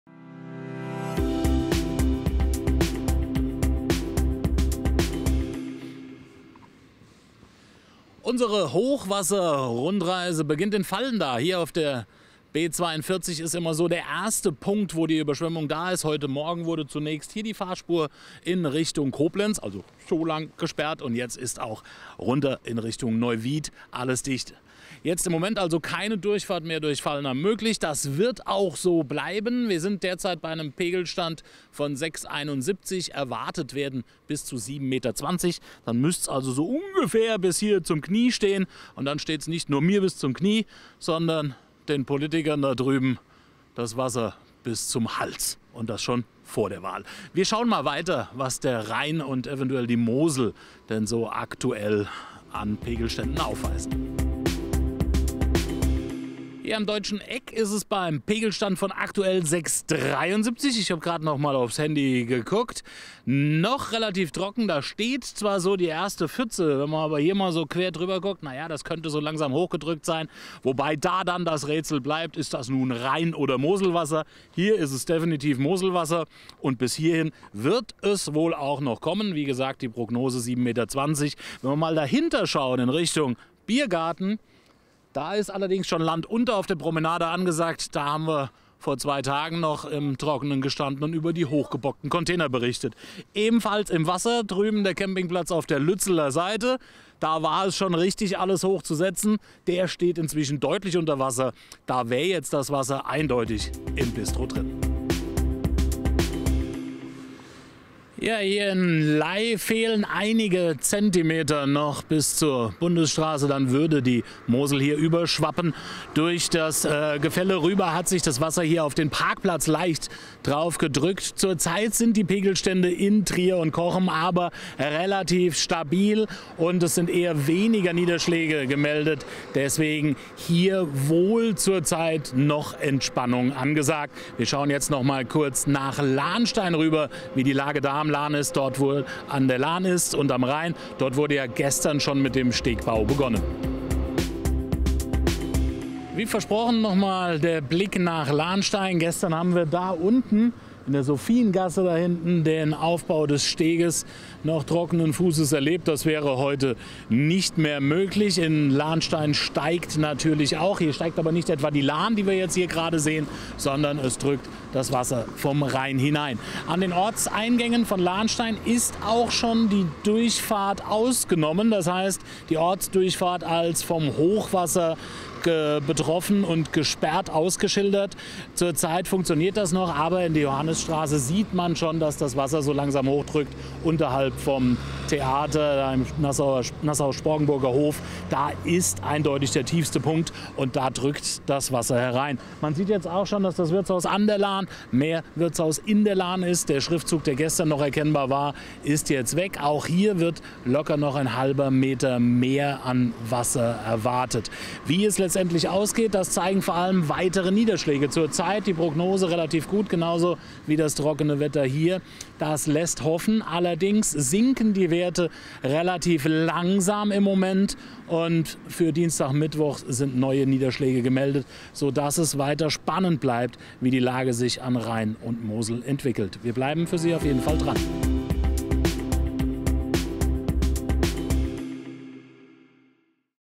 Hochwasser in der Region Koblenz | 1.2.2021
Hochwasser in der Region - Chefreporter Willi Willig zeigt euch auf einer kleinen Tour Eindrücke der derzeitigen Pegelstände rund um Koblenz.